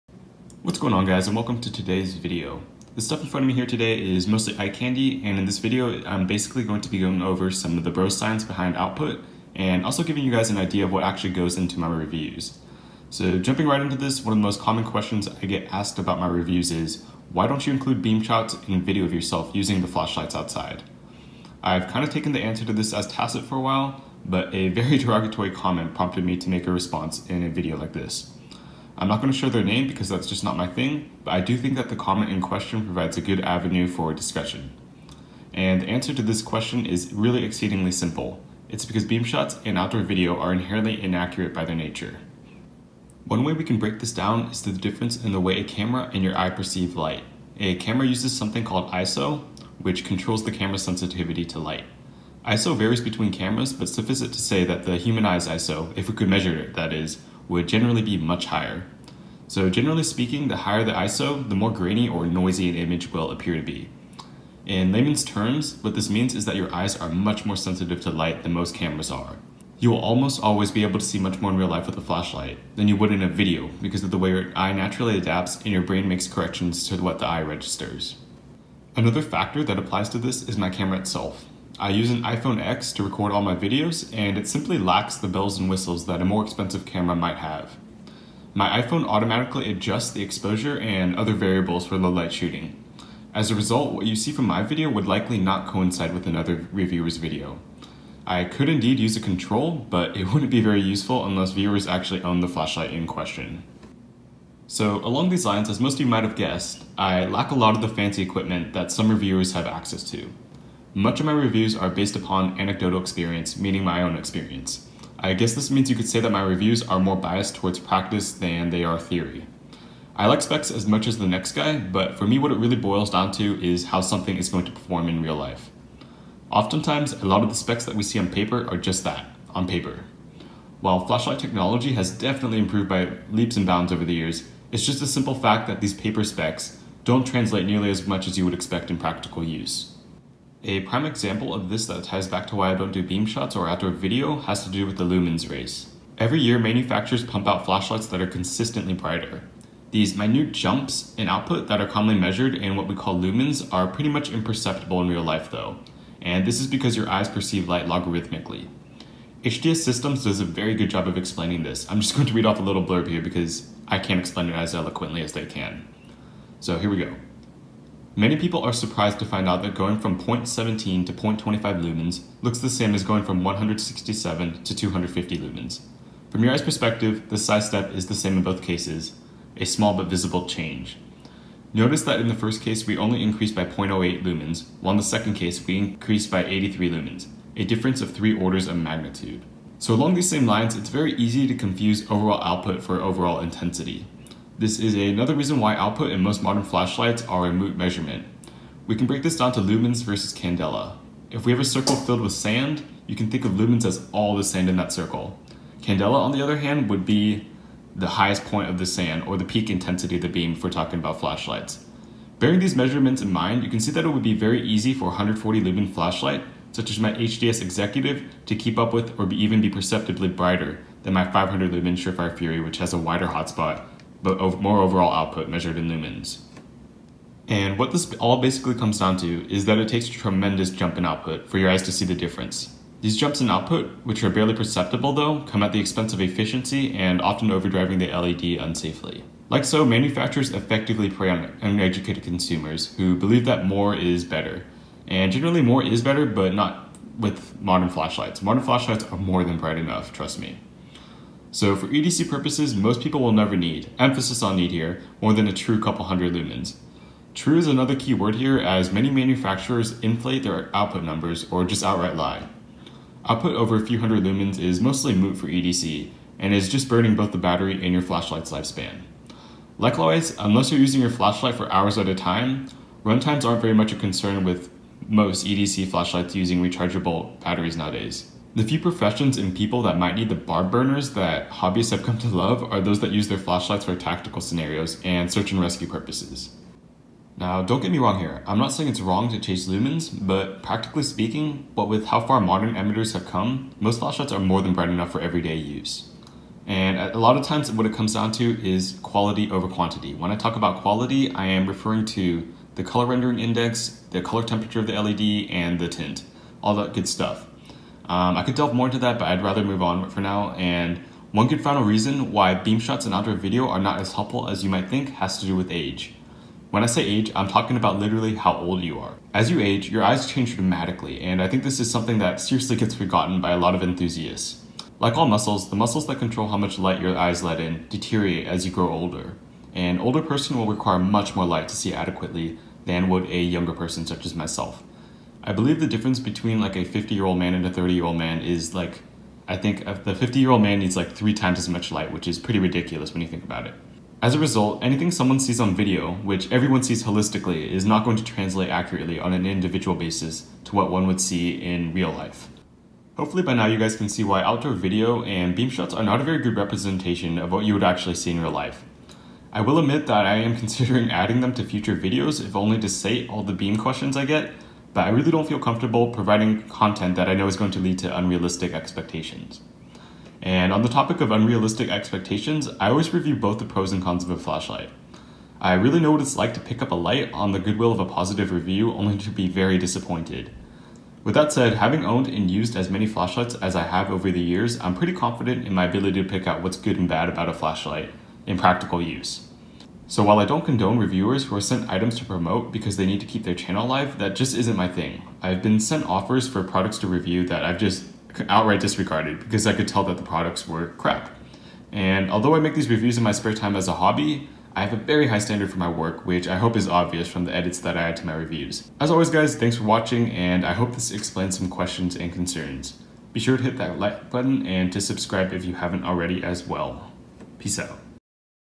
Flashlight Reviews: Why I Don’t Do Beamshots, Common Misconceptions, and Philosophy
I've been meaning to make a video on this question for a while now, but only recently did I feel compelled to because of a comment I received on one of my videos. So why don't I do beamshots and outdoor video? The answer, simply put, is that they are inherently inaccurate. We can break this down into a few main reasons.

Foremost is the difference in the way a camera and your eye perceive light. A camera uses something called ISO which controls the camera’s sensitivity to light. ISO varies between cameras, but suffice it to say that the human eye’s ISO - if we could measure it that is - would generally be much higher. In layman’s terms, your eyes are much more sensitive to light than most cameras are. You will almost always be able to see much more in real life with a flashlight than you would in a video because of the way your eye naturally adapts and your brain makes corrections to what the eye registers.

Second, output is often exaggerated or outright made-up by manufacturers. Every year, these manufacturers pump out flashlights that are consistently just a little brighter. These minute jumps in output that are commonly measured in lumens are pretty much imperceptible in real life, though, and this is because your eyes perceive light logarithmically. From HDS Systems: “Many people are surprised to find out that going from 0.17 to 0.25 lumens looks the same as going from 167 to 250 lumens. From your eye's perspective, the step size is the same in both cases - a small but visible change. Notice that in the first case we only increased by 0.08 lumens while in the second case we increased by 83 lumens - a difference of 3 orders of magnitude.”

To add to the confusion, it's also very easy to confuse overall output for intensity. This is another reason why output in most modern flashlights are a moot measurement. We can break this down to lumens versus candela. If we have a circle filled with sand, you can think of lumens as all the sand in the circle. Candela, on the other hand, is the highest point of the sand or the peak intensity of the beam. Bearing these measurements in mind, you can see that it would be very easy for a 140 lumen flashlight, such as my HDS Executive, to keep up with or even be perceptibly brighter than my 500 lumen Surefire Fury.

What this all basically comes down to is that it takes a tremendous jump in output for your eyes to see the difference, and this difference is just as hard to capture on video. These jumps in output which are barely perceptible, though, come at the expense of efficiency and often overdriving the LED unsafely. Manufacturers effectively prey on uneducated consumers like so, who believe that “more” is better.

Finally, beamshots and outdoor video are not as helpful as you might think because of age. When I say age, I’m talking about literally how old you are. As you age, your eyes change dramatically. And like all muscles, the muscles that control how much light your eyes let in deteriorate as you grow older. An older person will require much more light to see adequately than would a younger person such as myself. As a result, anything someone sees on video, which everyone sees holistically, is not going to translate accurately on an individual basis to what they would see in real life.

Hopefully you guys can see why outdoor video and beamshots are not a very good basis for video reviews. I’ll admit that I’m considering adding them to future videos if only to sate all the beam questions I get, but I really don’t feel comfortable providing content that I know is going to lead to unrealistic expectations.